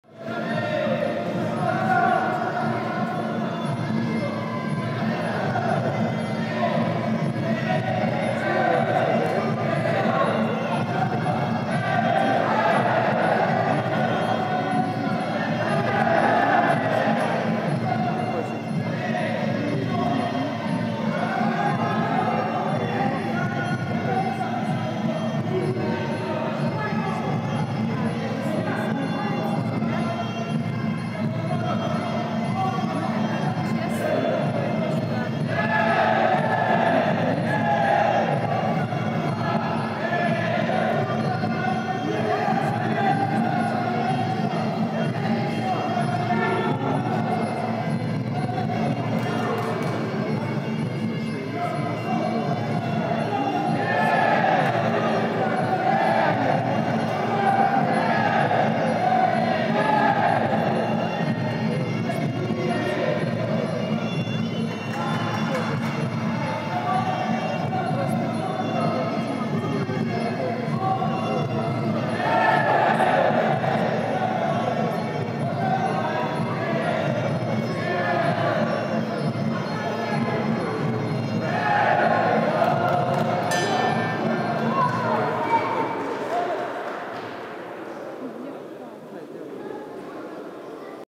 Макеев Даниил Бангкок Окулич Александр Лао Тай 14 15 лет 46 кг финал Ч1
Тайский Бокс г. Ломоносов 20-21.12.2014